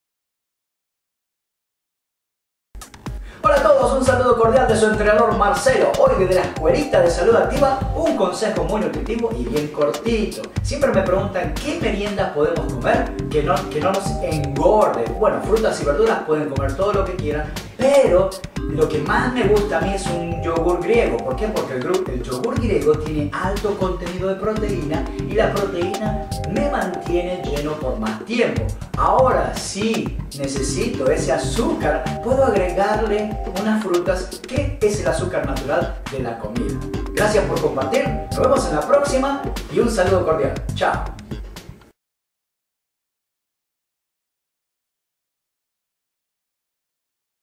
S.A.TV #108: Como Reducir la Cintura -Tip #3/How to Reduce the Waistline.
Adelgaza YA! De esta forma nos motivas a seguir haciendo más videos para ti! Despliega para más información**

Hola gente activa! En este episodio te comparto un tip muy práctico y eficaz si lo que quieres es reducir tu cintura. Las meriendas son comidas pequeñas que te ayudan a continuar el día sin pasa hambre hasta tu próxima comida. Una de mis meriendas favoritas es un yogurt Griego, porque tiene alto contenido de proteína, lo cual me mantendrá lleno por más tiempo, y es fácil de digerir. Ahora si le quieres agregar más sabor, la adición de frutas es una muy buena opción. No te olvides que las frutas contienen azúcar natural, y este azúcar es transformado en energía en el cuerpo. Saludos!

**Te Recomiendo Estos Productos**

Mi Libro de Rutinas "Quema-Grasa" @